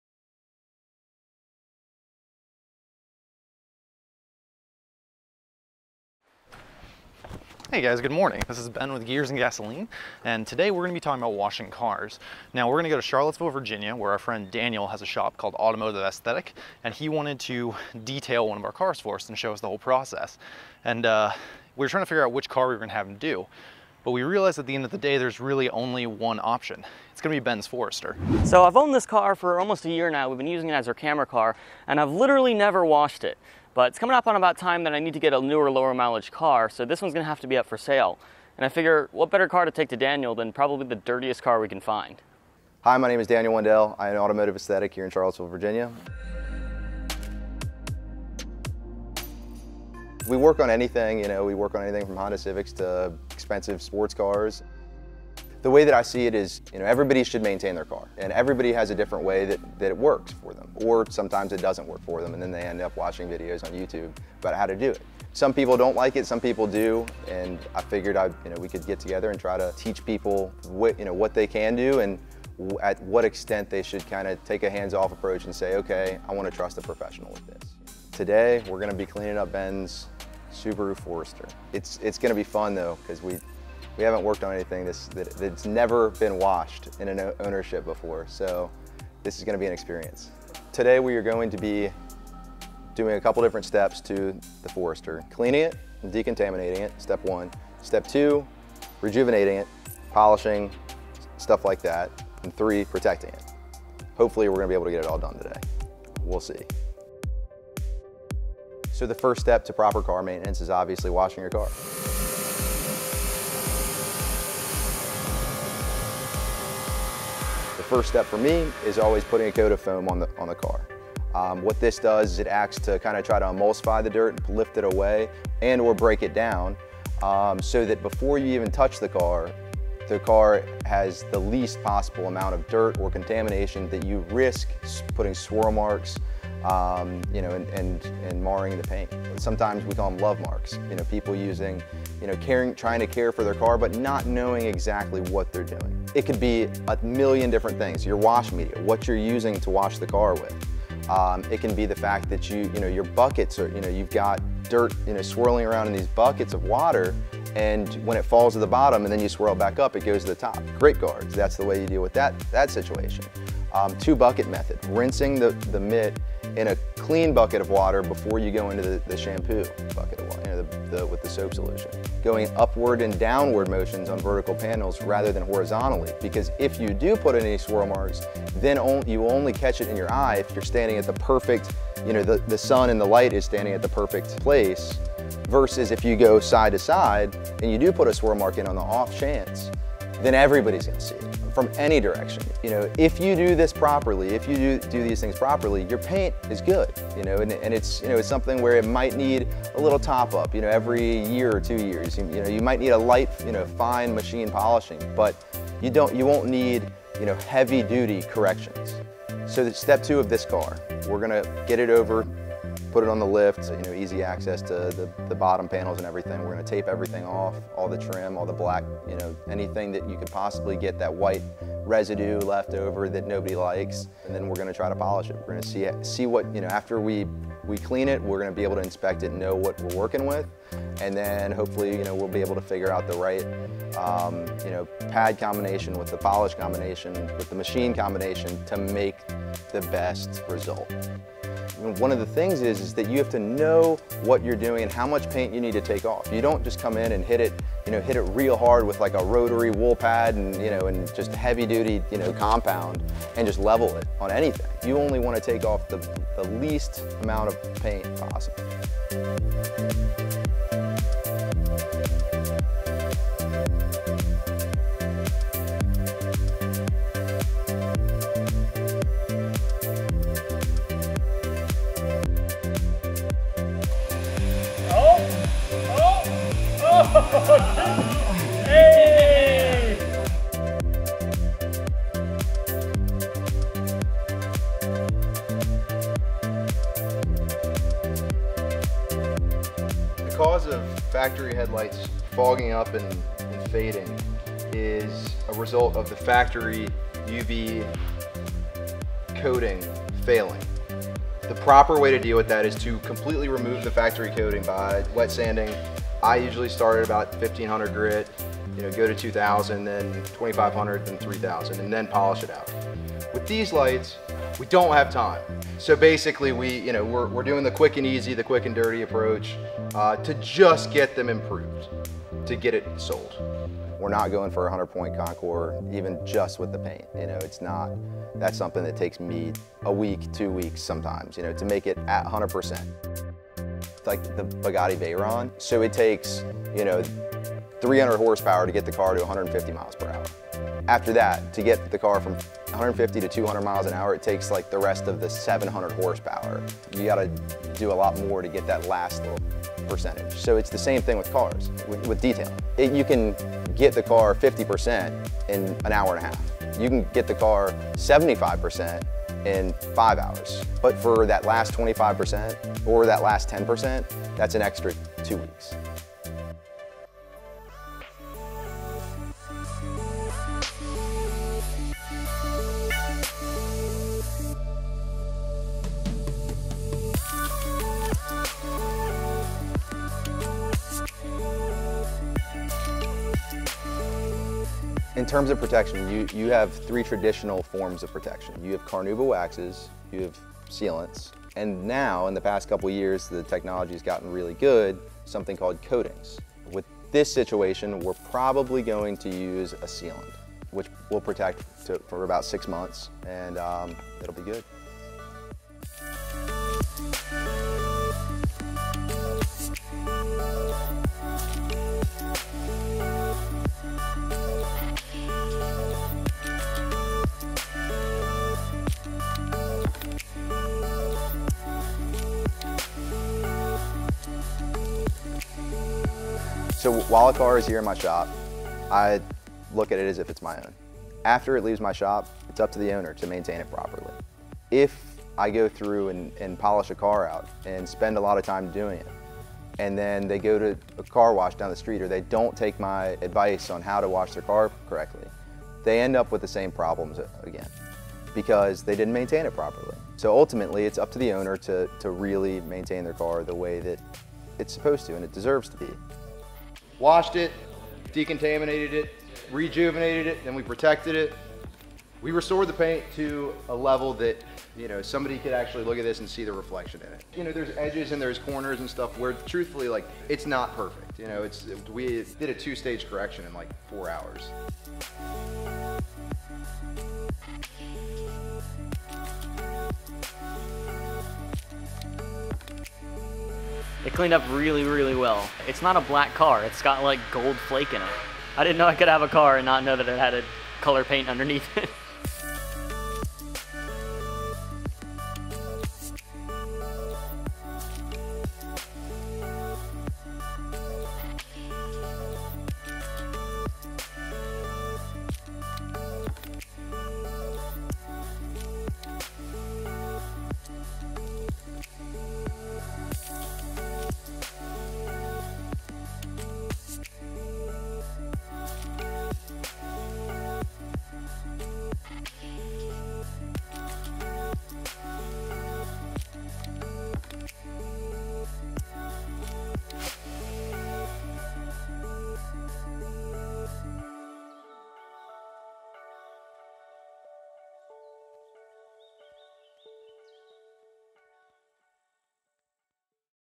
Detailing a 200,000 Mile Car
Ben bought the Forester last year to serve as a beater, and we have certainly beat on it. The car hasn't been washed by anything but rain for the entirety of Ben's ownership, so when our buddy Daniel at Automotive Aesthetic offered to detail a car for us, we had the perfect challenge for him.

In this video, Daniel talks through his 3 main steps of cosmetic car care, and cleans up the Subie while he's at it.

Huge thanks to Daniel and all the guys at Automotive Aesthetic for hooking us up with an unreasonably great detail.

and @gearsandgasoline